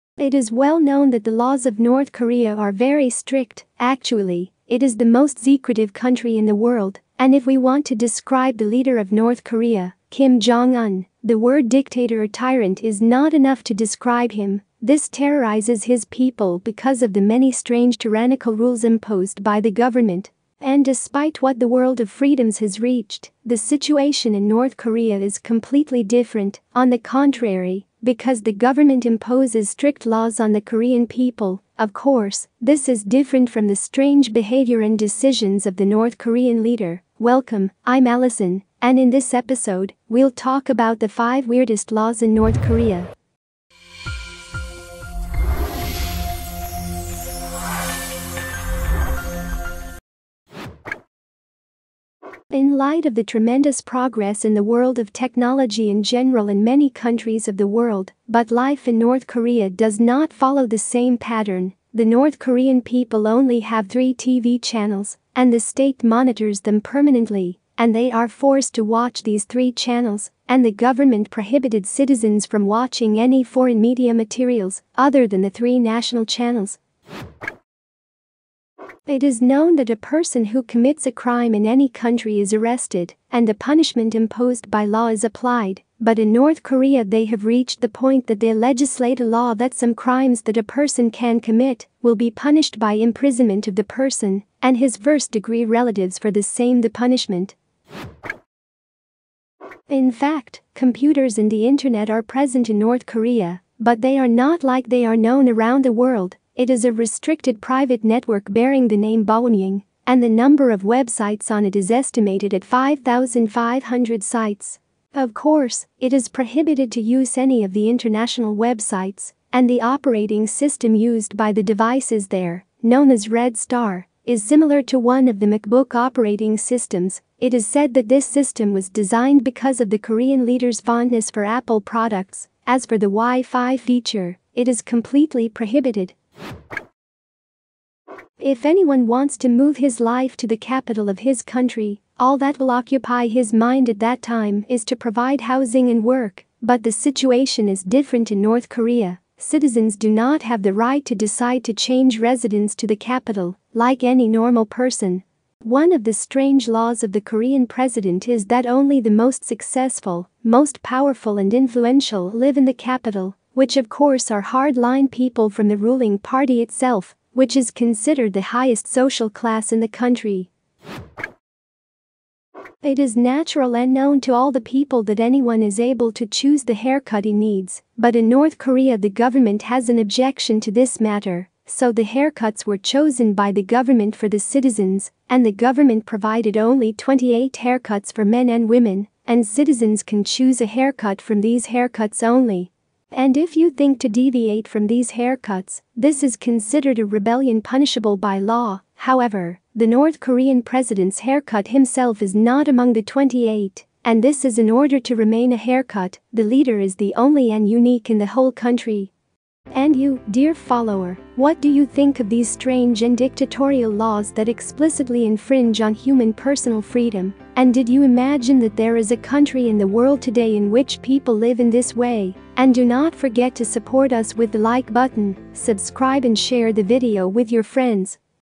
Five strange laws in North Korea
In this video, we will talk about the five strangest laws in North Korea.. You will not believe that there are people today who live their lives in this way.. They are exposed to dictatorship and class discrimination to this degree.